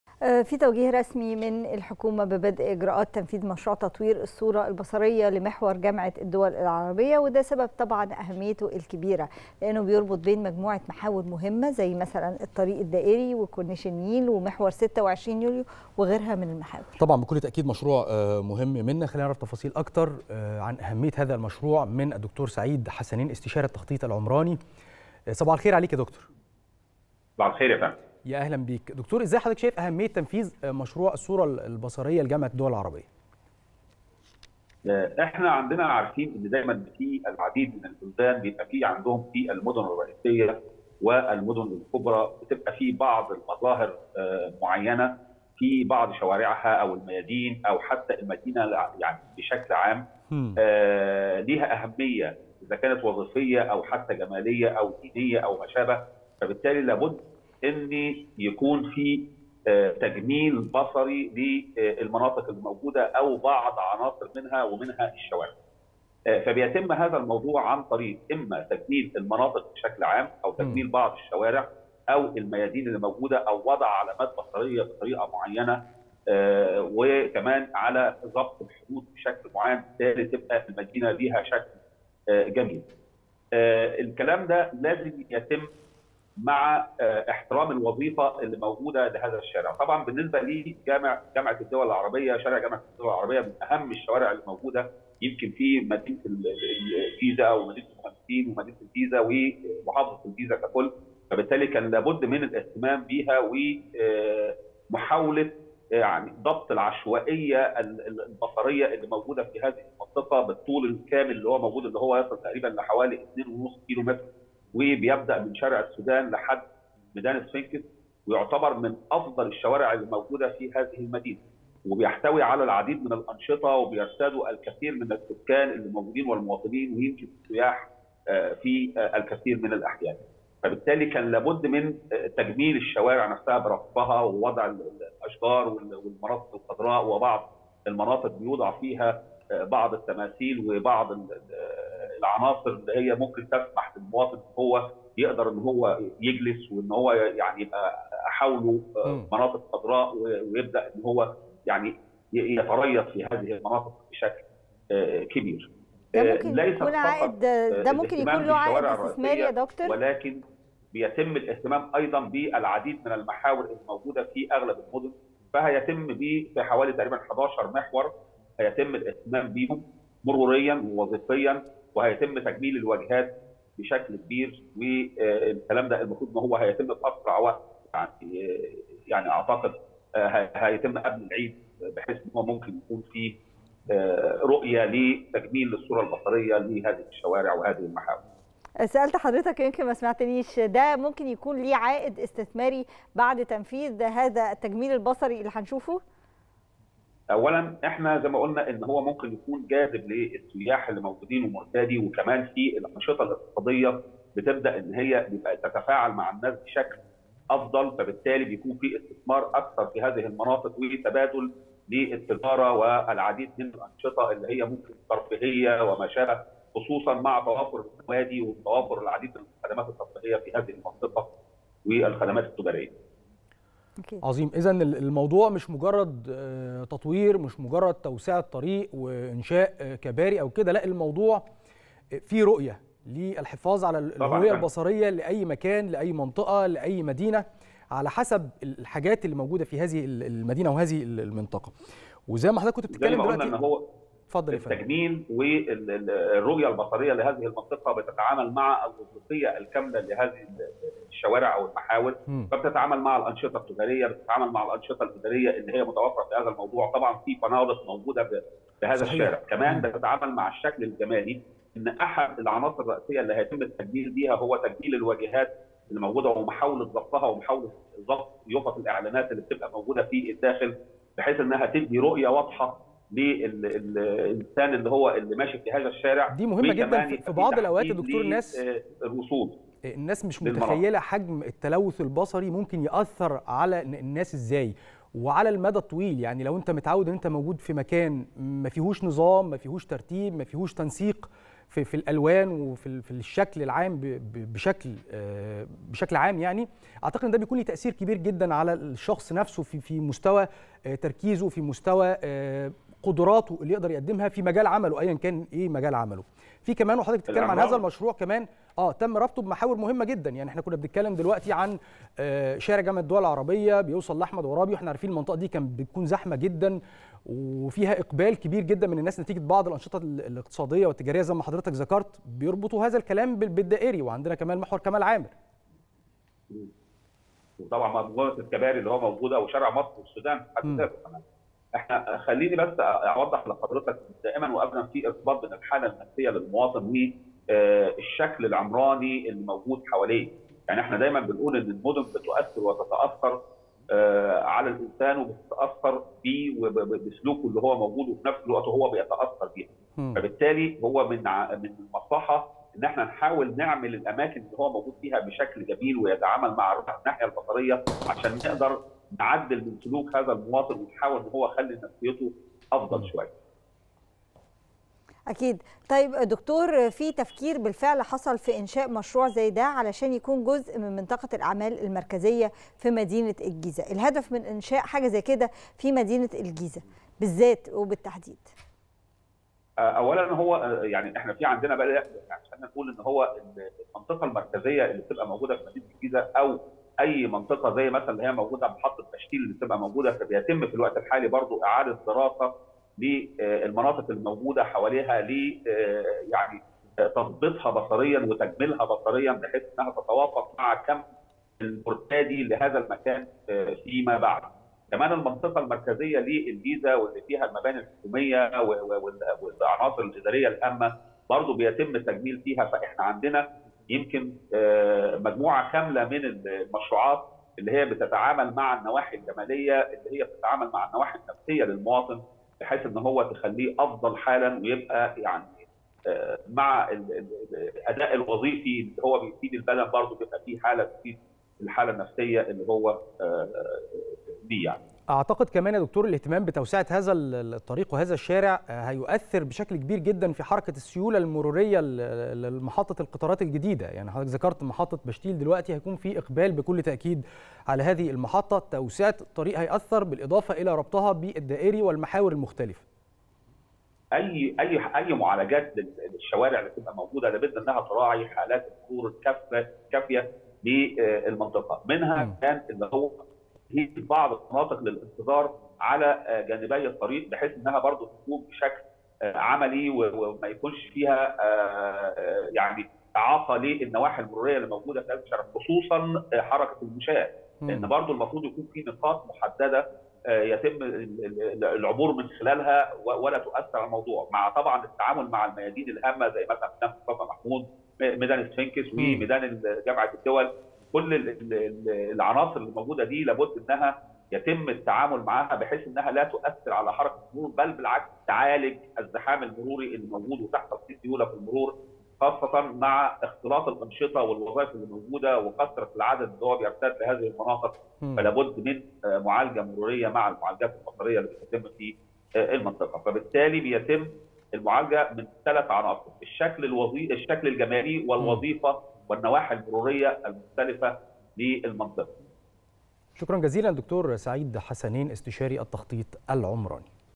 تعرف على التفاصيل الكاملة لتطوير الصورة البصرية لمحور جامعة الدول العربية .. من د.سعيد حسانين
تعرف على التفاصيل الكاملة لتطوير الصورة البصرية لمحور جامعة الدول العربية .. من استشاري التخطيط العمراني د.سعيد حسانين